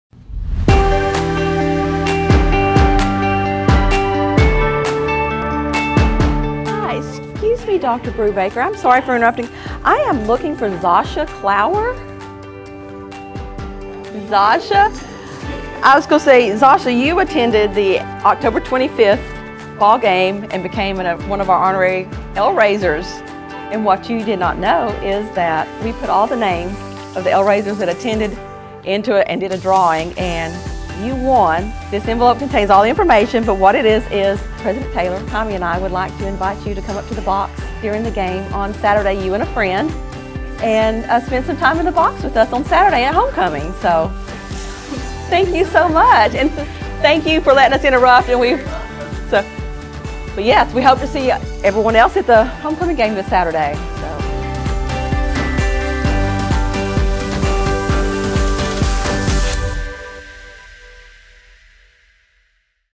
Lamar University L-Raiser Special Invitation
In honor of the start of Homecoming week, First Lady Taylor delivered a special invitation to one lucky L-Raiser this morning.